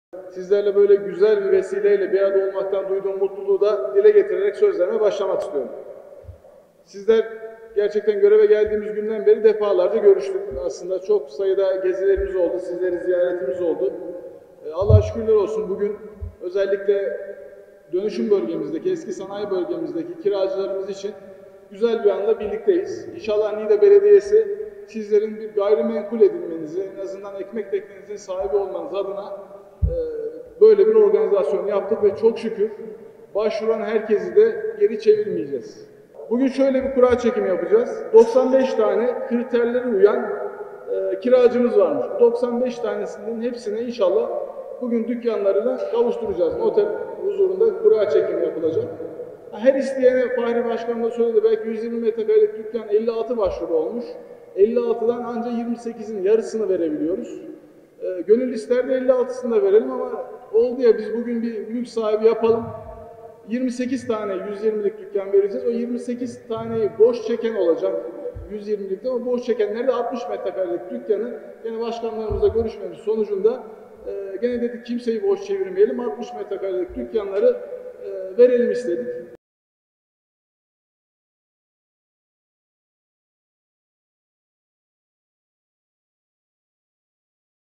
Yeni Sanayi Sitesinden 95 Dükkan Sahibi Kura İle Belirlendi
Niğde Belediyesi’nin sanayi dönüşüm projesinde hak sahiplerine verilecek dükkanlarının belirlenmesi için noter huzurunda kura çekildi.